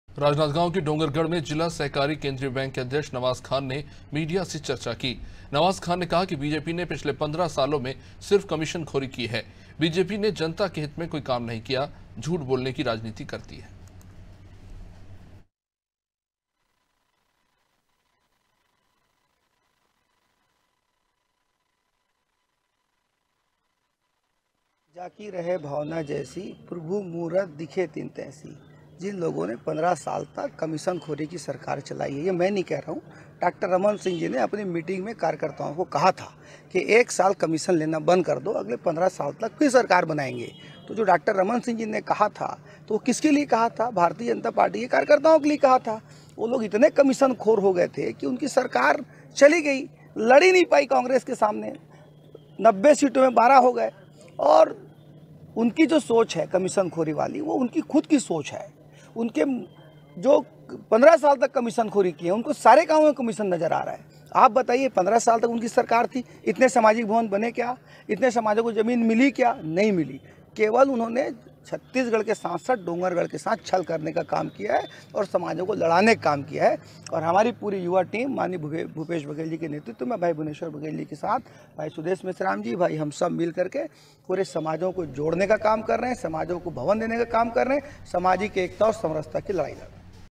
Rajnandgaon : नवाज खान का बीजेपी पर निशाना बीजेपी करती है कमीशनखोरी || Anaadi Tv
राजनांदगांव के डोंगरगढ़ में जिला सहकारी केंद्रीय बैंक के अध्यक्ष नवाज खान ने मीडिया से चर्चा की है...नवाज खान ने कहा कि बीजेपी ने पिछले 15 सालों में सिर्फ कमीशनखोरी की है...बीजेपी ने जनता के हित में कोई काम नहीं किए हैं...बीजेपी झूठ बोलने की राजनीति करती है...

COMING SOON
 DEN, AIRTEL